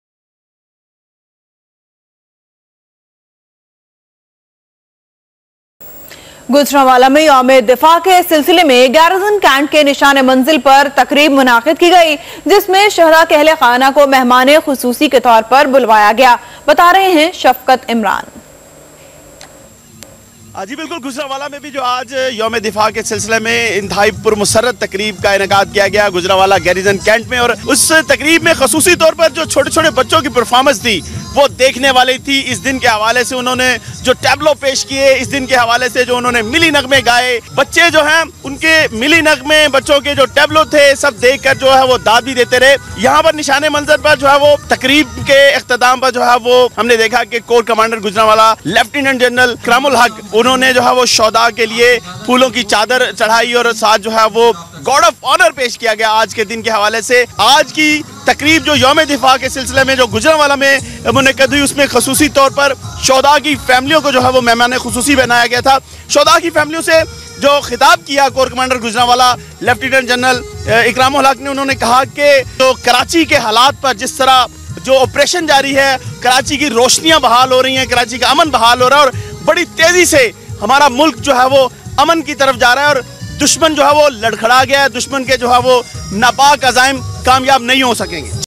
Gujranwala Cantonment: Lt. General Ikram ul Haq pays homage to martyrs on Defence Day
24 News HD is one of the leading news channels of Pakistan bringing you the latest current affairs from Pakistan and around the world.